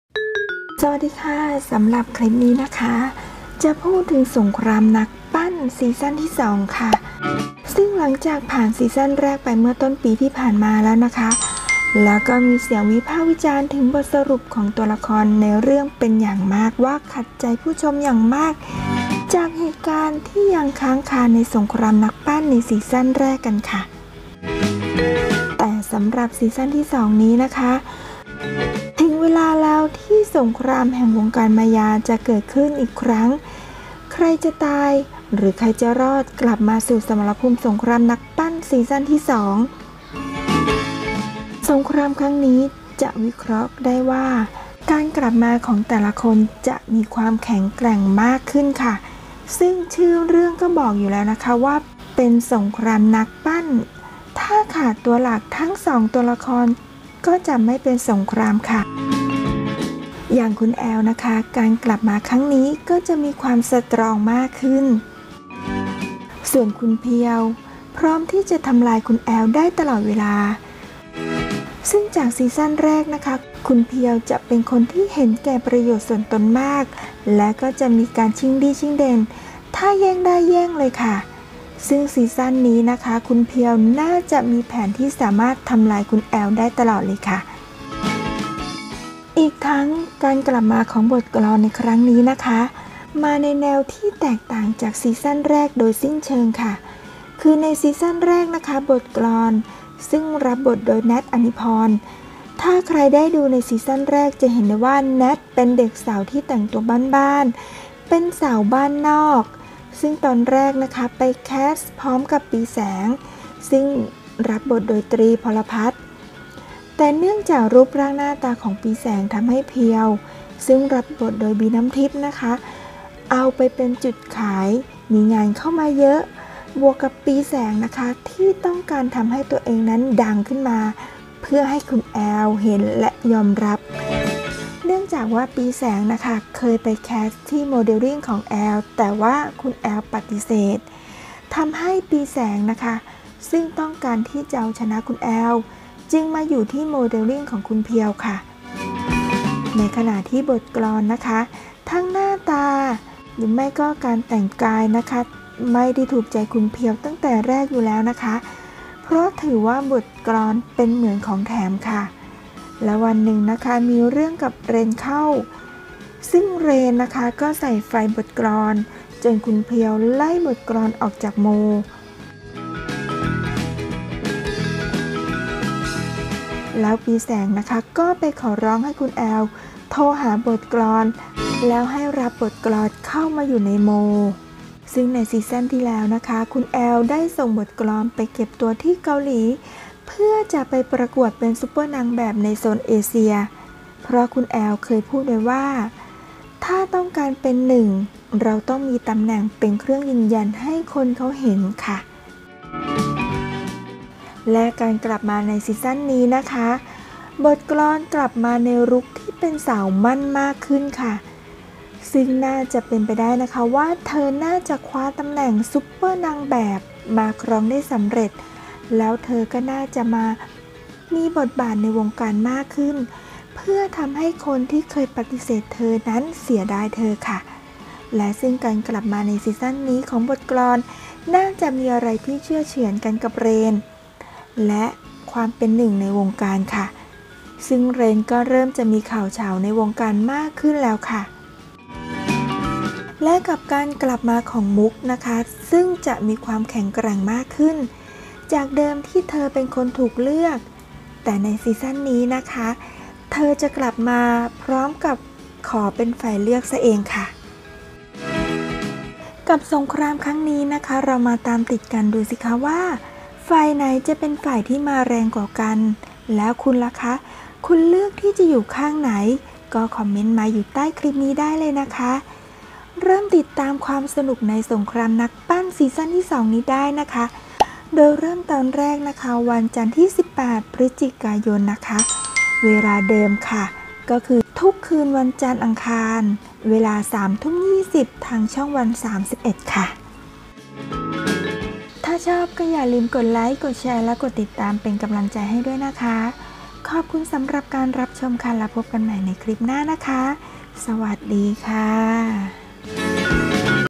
กลับมาเพื่อไขความลับที่ค้างคาใจกับ สงครามนักปั้น season 2 ใครจะอยู่หรือใครจะดับมาดูกัน
สวัสดีค่ะ สำหรับคลิปนี้ จะพูดถึงสงครามนักปั้น season2 ซึ่งหลังจากผ่านซีซั่น แรกไปเมื่อต้นปีที่ผ่านมาและมีเสียงวิพากษ์วิจารณ์ถึงบทสรุปของตัวละครในเรื่องเป็น
อย่างมาก ว่าขัดใจผู้ชมอย่างมากจากเหตุการณ์ที่ยังค้างคาในสงครามนักปั้นใน season แรก
แต่สำหรับ season ที่ 2 นี้ ถึงเวลาที่สงครามแห่งวงการมายา จะเกิดขึ้นอีกครั้ง  ใครจะตาย   หรือใครจะรอดกลับมาสู่สมรภูมิสงครามนักปั้น season ที่ สอง
สงครามครั้งนี้ จะวิเคราะห์ได้ว่า การกลับมาของแต่ละคนจะมีความแข่งแกร่งมากขึ้น ซึ่งชื่อเรื่องก็บอกแล้วค่ะ ว่าเป็นสงครามนักปั้น ถ้าขาด ตัวหลัก
ทั้ง 2 ตัวละครก็จะไม่เป็นสงครามค่ะ  อย่าง
คุณแอล จะกลับมา มีความสตรองมากขึ้น
คุณเพียว พร้อมที่จะทำลายคุณแอลได้ตลอดเวลา ซึ่งจาก season แรก คุณเพียว จะเป็นคนที่เห็นแก่ประโยชน์ส่วนตนมาก และจะชิงดีชิงเด่น ถ้าแย่งได้ แย่ง
ซึ่ง season นี้ คุณเพียว น่าจะมีแผนที่สามารถทำลาย แอลได้ตลอดเลยค่ะ
อีกทั้งการกลับมาของบทกลอนในครั้งนี้ มาในแนวที่แตกต่างจาก season แรก โดยสิ้นเชิง คือ ใน season แรก บทกลอน ซึ่งรับบทโดยแนท อนิพรณ์
ถ้าใครได้ดูใน season แรกจะเห็นได้ว่า แนท เป็นเด็กสาวแต่งตัวบ้านๆ เป็นสาวบ้านนอก ซึ่งตอนแรก ไปแคสพร้อมกับปีย์แสง ซึ่งรับบทโดยตรี ภรภัทร เนื่องจากรูปร่าง
หน้าตาของปีแสงทำให้ เพียว ซึ่งรับบทโดย บี น้ำทิพย์ เอาไปเป็นจุดขาย มีงานเข้ามาเยอะ บวกกับปีแสง ต้องการทำให้ตนเองดังเพื่อให้คุณแอลเห็นและยอมรับเนื่องจาก
ปีแสงเคยไปแคสที่ โมเดลลิ่งของคุณแอลแต่คุณแอลปฏิเสธ ทำให้ปีแสงซึ่งต้องการเอาชนะคุณแอล จึงมาอยู่โม ของคุณเพียวค่ะ ในขณะที่ บทกลอน ทั้งหน้าตา ไม่การแต่ง
กาย ไม่ได้ถูกใจคุณเพียวตั้งแต่แรกอยู่แล้ว เพราะถือว่า บทกลอนเป็นเหมือนของแถม และวันนึงมีเรื่องกับเรนเข้า ทำให้เรนใส่ไฟ บทกลอน จนคุณเพียว ไล่บทกลอนออกจาก
โม แล้ว ปีแสง ก็ไปขอร้องให้คุณแอลโทรหาบทกลอน แล้วให้รับบทกลอนเข้าโม ซึ่งใน season ที่แล้ว คุณแอลได้ส่งบทกลอนไปเก็บตัวที่เกาหลีเพื่อจะไปประกวด
super นางแบบในโซนเอเชีย เพราะคุณแอล เคยพูดไว้ว่า ถ้าต้องการเป็นหนึ่งเราต้องมีตำแหน่ง เป็นเครื่องยืนยันให้คนเขาเห็น
ซึ่งใน season นี้ บทกลอนกลับมาในรุกที่เป็นสาวมั่นมากขึ้น ซึ่งน่าจะเป็นไปได้ว่าเธอน่าจะคว้าตำแหน่ง super นางแบบมาครองได้สำเร็จ แล้วเธอน่าจะเข้ามามีบทบาทในวงการมากขึ้น
เพื่อทำให้คนที่เคยปฏิเสธเธอนั้นเสียดายเธอ ซึ่งการกลับมาใน season นี้ของ บทกลอน น่าจะเป็นอะไรที่เชือดเฉือนกันกับเรน และความเป็นหนึ่งในวงการ ซึ่ง
เรนก็เริ่มจะมีข่าวฉาวในวงการมากขึ้นแล้ว ค่ะ
 กลับการกลับมา มุก จะมีความแข็งแกร่งมากขึ้น จากเดิมที่เธอเป็นคนถููกเลือก แต่ part นี้การกลับมาของเธอจะขอเป็นฝ่ายเลือกซะเอง

สงครามครั้งนี้ เรามาตามติดกันสิคะว่า ฝ่ายไหนจะเป็นฝ่ายที่มาแรงกว่ากัน คุณเลือกข้างไหน ก็คอมเมนต์มาใต้คลิปนี้ได้เลยนะคะ เริ่มติดตามความสนุกในสงครามนักปั้น
season 2 นี้ได้เริ่มตอนแรก 18 พฤศจิกายน เวลาเดิมคือ ทุกวัน จันทร์ อังคาร เวลา 21.20 น. ทางช่องวัน 31 ค่ะ
ชมแล้วอย่าลืมกดติดตามให้แม่อ้อยด้วยนะคะ เพื่อเป็นกำลังใจในการทำคลิปต่อไปค่ะ  และขอบคุณสำหรับการรับชมนะคะ

ติดต่อพูดคุยกันได้ที่